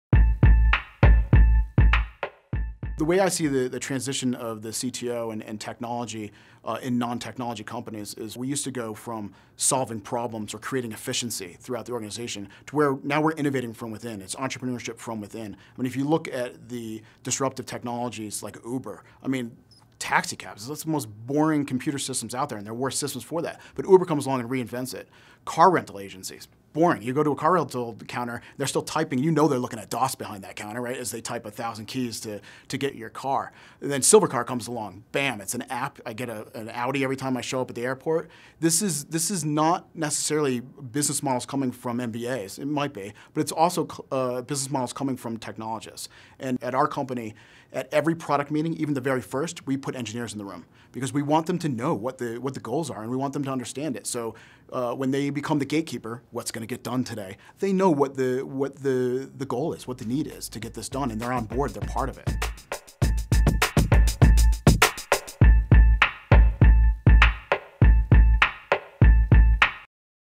Entrepreneurship from Within
How IT is moving from solving problems to driving innovation.